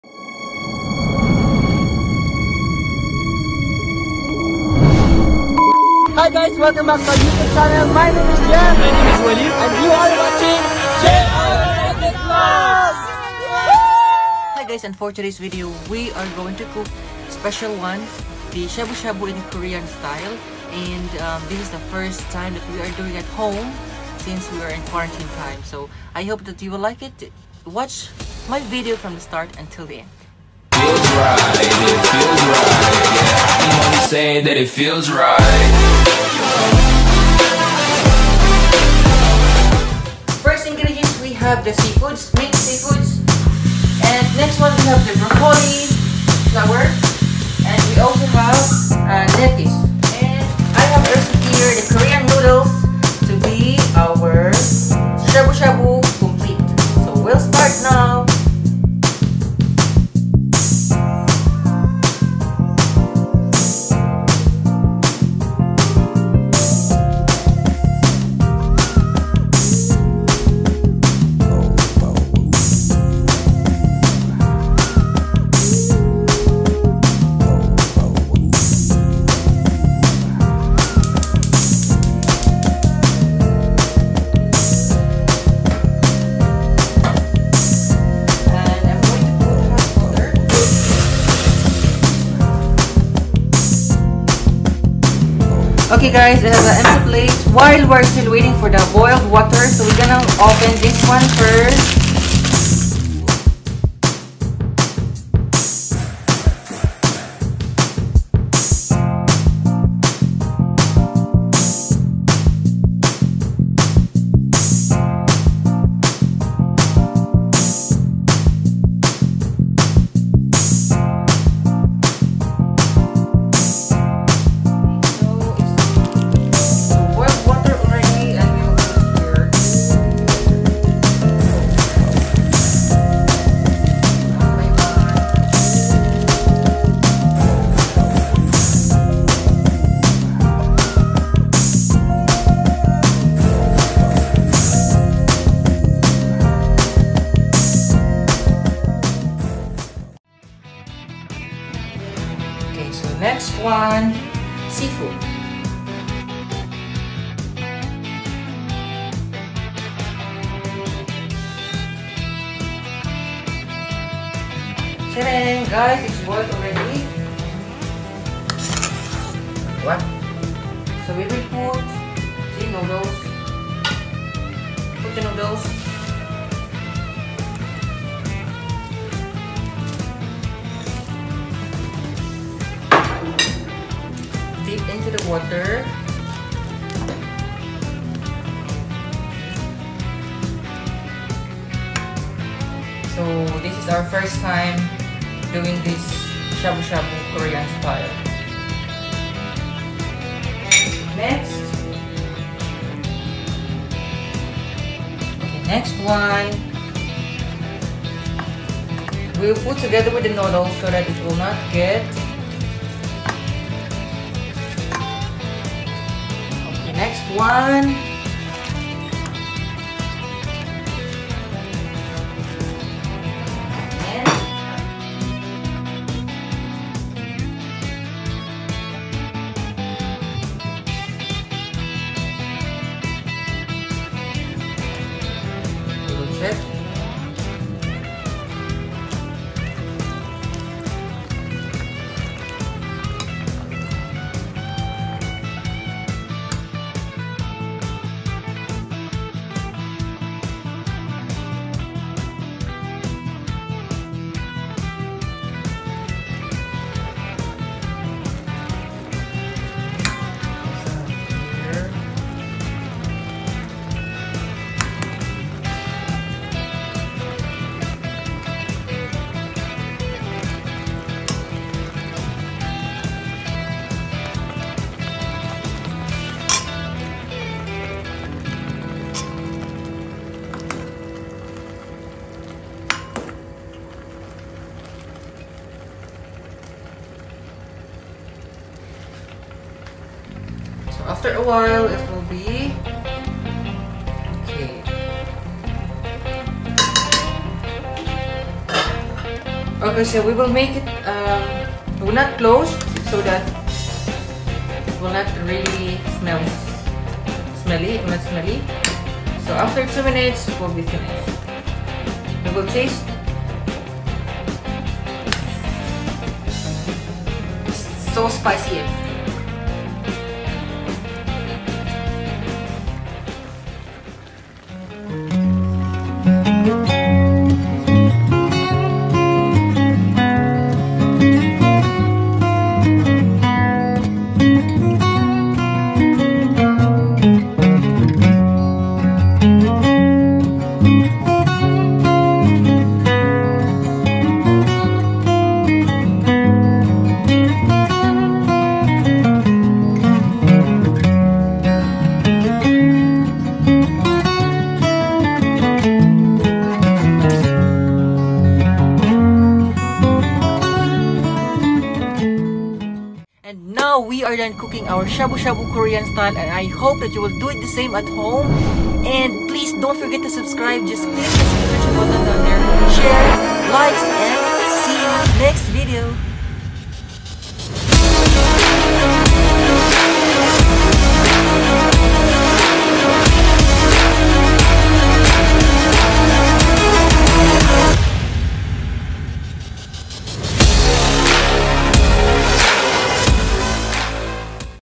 HOW TO MAKE SHABU—SHABU AT HOME| FOOD PORN| KOREAN STYLE
shabu-shabu a Japanese dish consisting of thinly sliced beef and vegetables cooked briefly in simmering broth at the table. But for our very own version we used seafoods instead of beef or pork.  It is really good and healthy food. Making it here only inside out lil home. Due to have a lockdown and facing This pandemic situation.